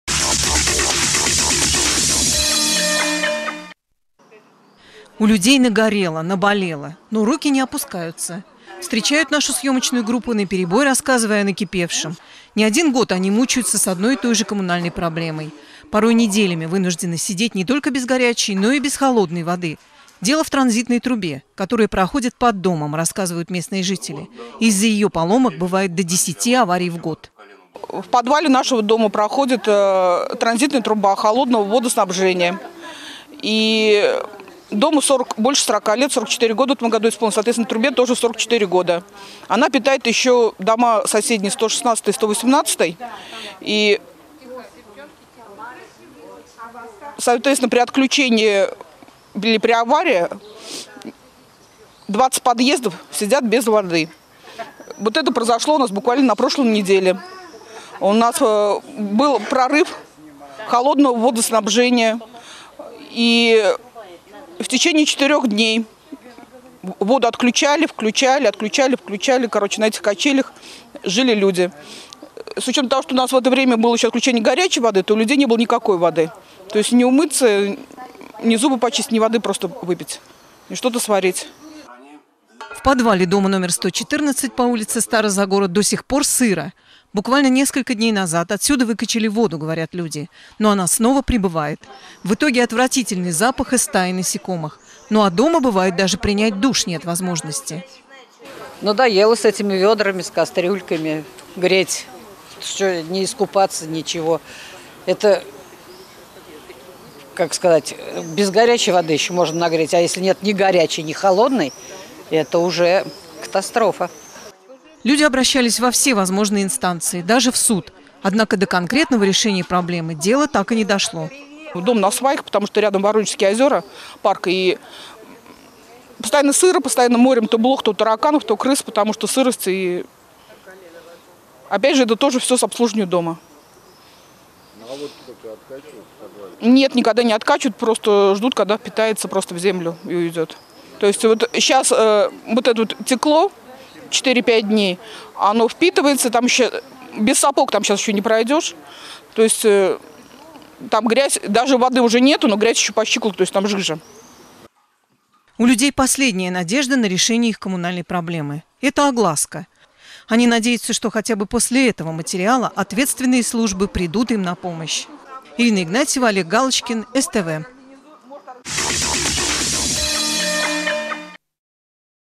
Рубрика «Дежурный по городу» от 30 мая 2023
Отвратительный запах, стаи насекомых и сырость в подвале. А в квартирах, порой, нет воды по несколько дней. С такой проблемой столкнулись жители дома номер 114 по улице Стара Загора. В причинах разбирались и наши корреспонденты. Об очередной коммунальной беде из жизни Самарцев - в нашей постоянной рубрике "Дежурный по городу".